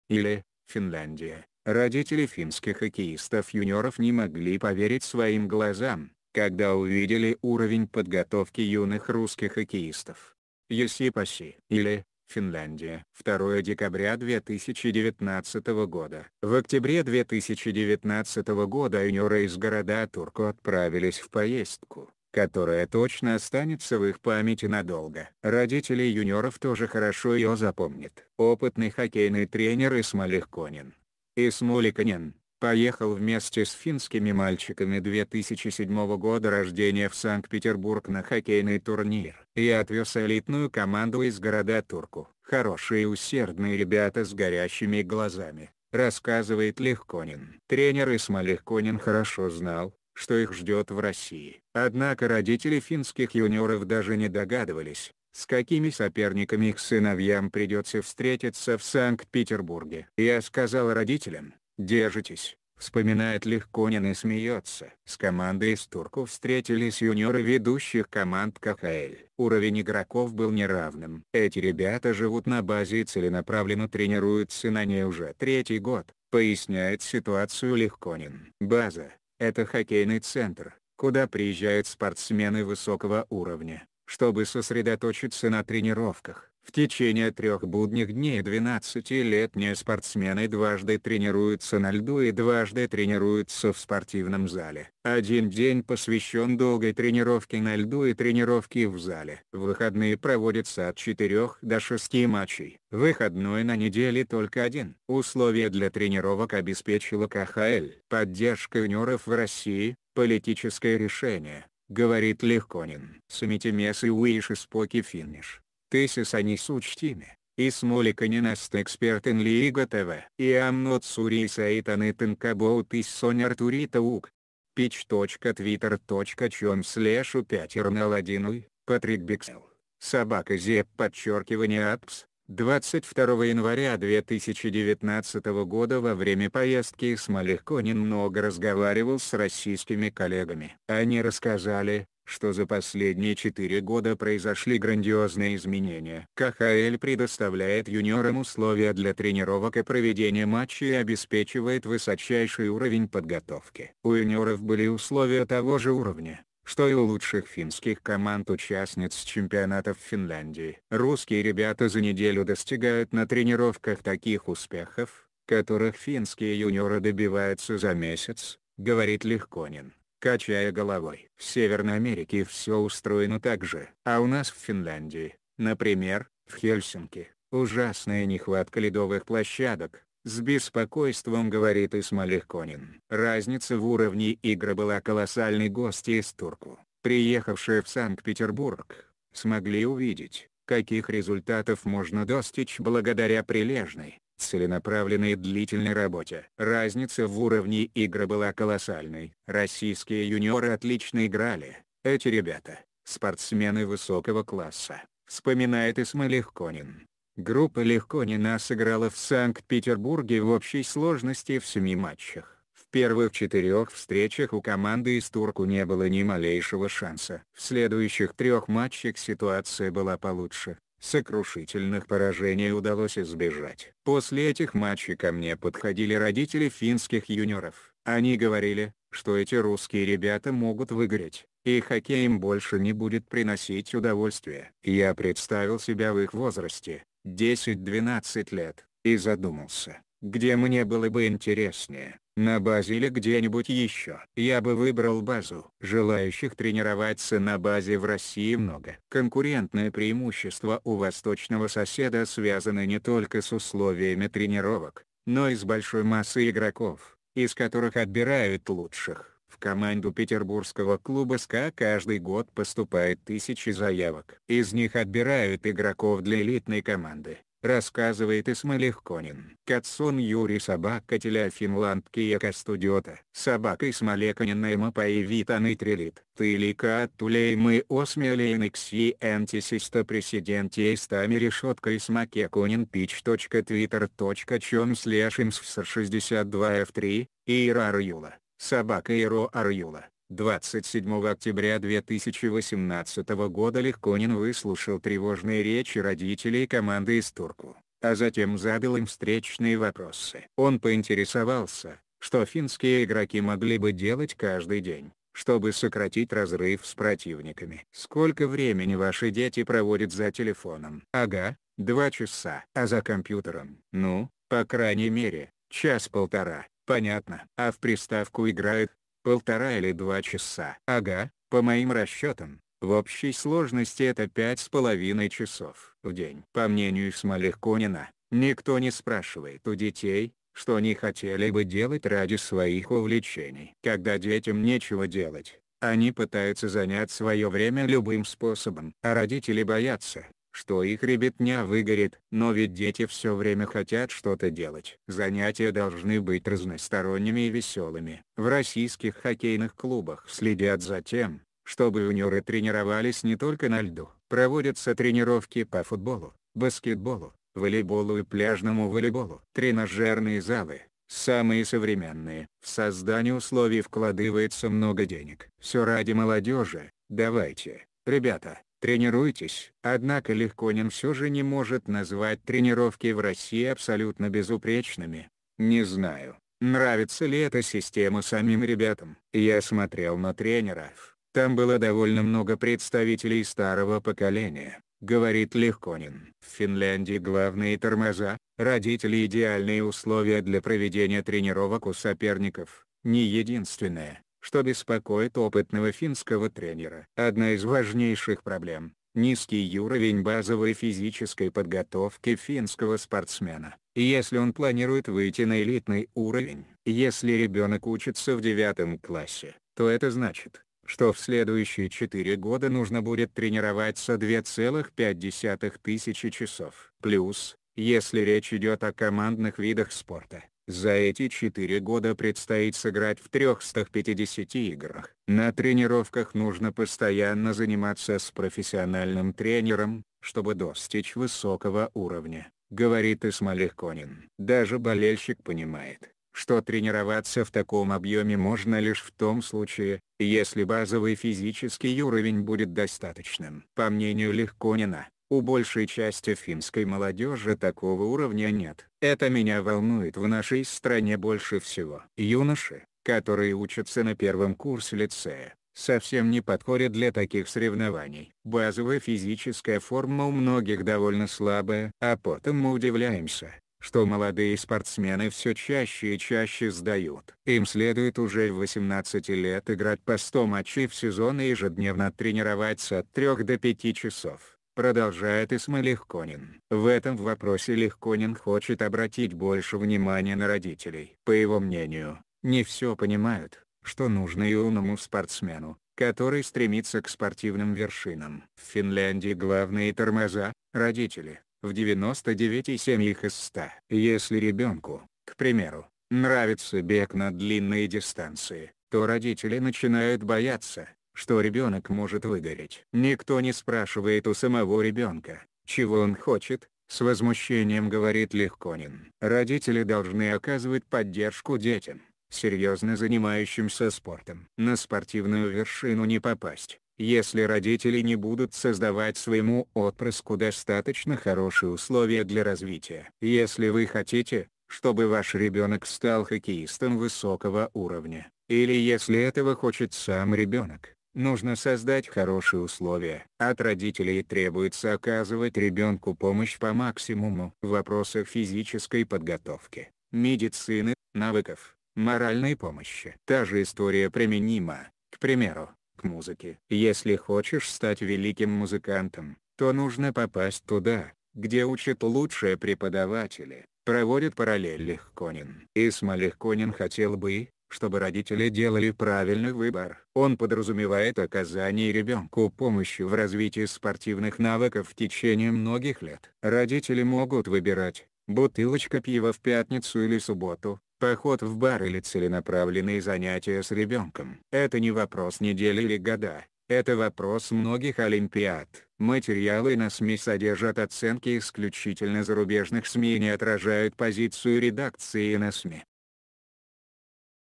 Yle (Финляндия): родители финских хоккеистов-юниоров не могли поверить своим глазам, когда увидели у
Yle (Финляндия): родители финских хоккеистов-юниоров не могли поверить своим глазам, когда увидели уровень подготовки юных русских хоккеистов.
Yle, Финляндия.
Юсси Пасси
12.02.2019

В октябре 2019 года юниоры из города Турку отправились в поездку, которая точно останется в их памяти надолго. Родители юниоров тоже хорошо ее запомнят.  Опытный хоккейный тренер Исмо Лехконен (Ismo Lehkonen) поехал вместе с финскими мальчиками 2007 года рождения в Санкт-Петербург на хоккейный турнир.  «Я отвез элитную команду из города Турку. Хорошие усердные ребята с горящими глазами», — рассказывает Лехконен. Тренер Исмо Лехконен хорошо знал, что их ждет в России. Однако родители финских юниоров даже не догадывались, с какими соперниками их сыновьям придется встретиться в Санкт-Петербурге.  «Я сказал родителям: „Держитесь!"», — вспоминает Лехконен и смеется.  С командой из Турку встретились юниоры ведущих команд КХЛ. Уровень игроков был неравным.  «Эти ребята живут на базе и целенаправленно тренируются на ней уже третий год», — поясняет ситуацию Лехконен.  База — это хоккейный центр, куда приезжают спортсмены высокого уровня, чтобы сосредоточиться на тренировках. В течение трех будних дней 12-летние спортсмены дважды тренируются на льду и дважды тренируются в спортивном зале. Один день посвящен долгой тренировке на льду и тренировке в зале. В выходные проводится от четырех до шести матчей. Выходной на неделе только один.  «Условия для тренировок обеспечила КХЛ. Поддержка юниоров в России — политическое решение», — говорит Лехконен.    Sometimes I wish I spoke Finnish. This is one such time, Ismo Lehkonen as the expert in Liiga TV. I am not sure he said anything about his s...